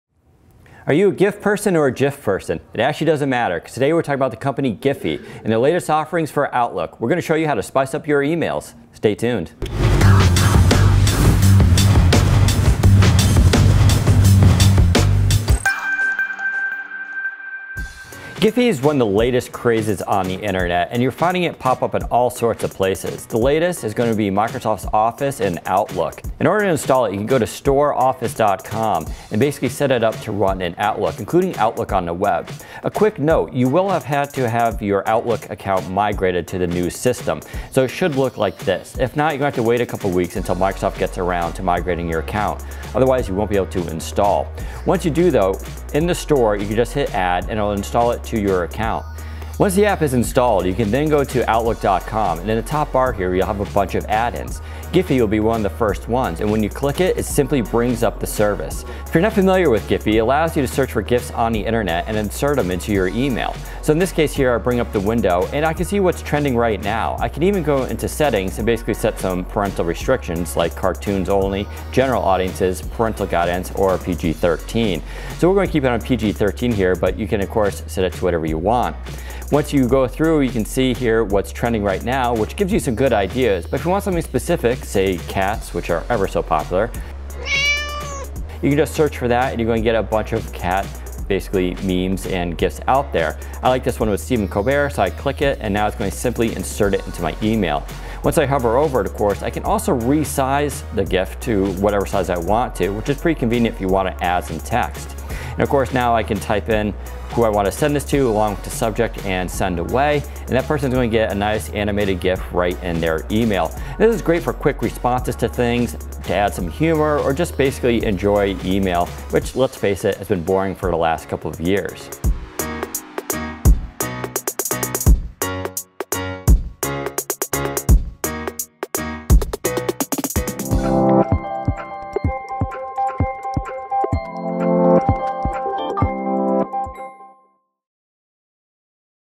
Giphy for Microsoft Outlook
Giphy is now available right in Microsoft Outlook so you can crank your emails up a notch by using Giphy to add fun gifs to your emails.